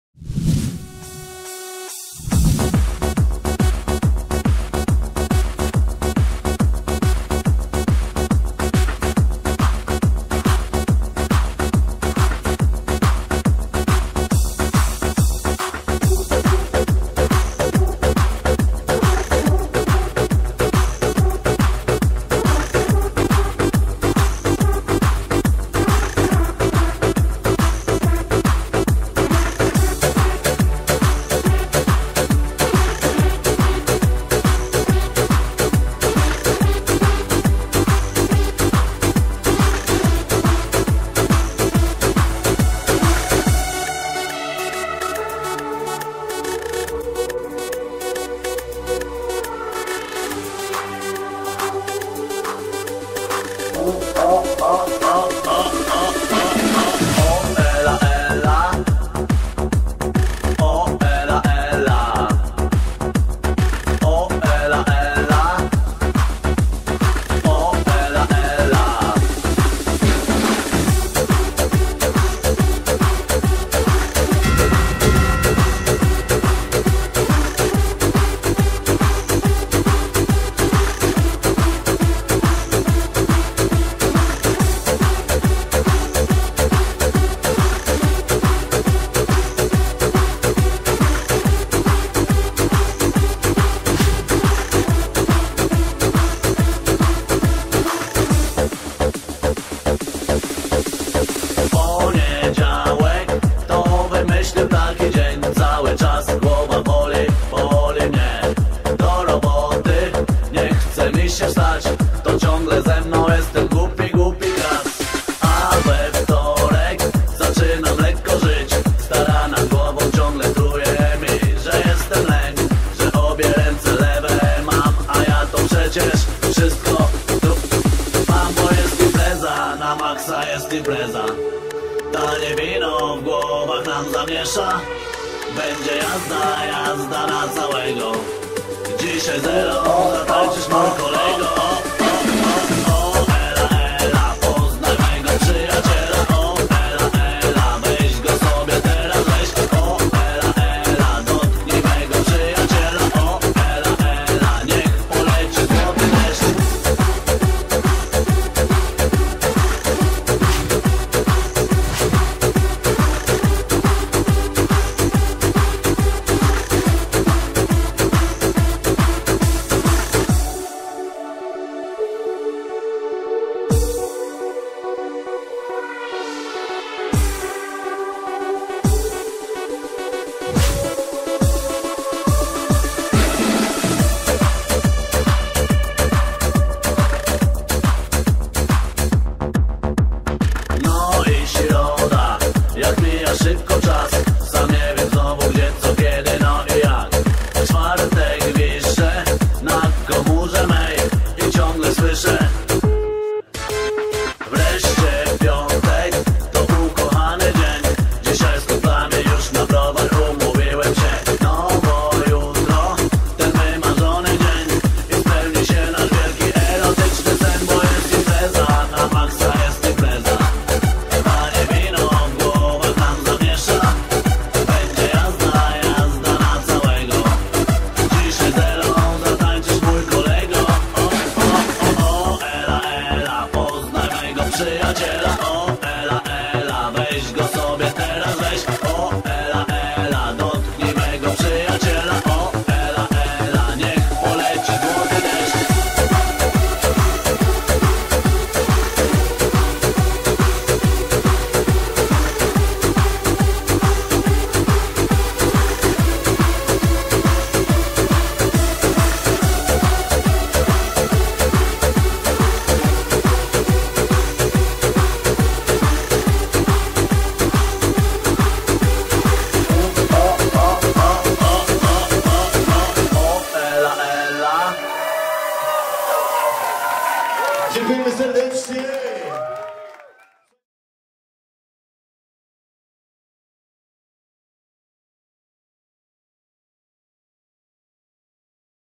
OSTATKI - Crazy Party, Dj Marcus & D-BOMB
gwiazda wieczoru: D-Bomb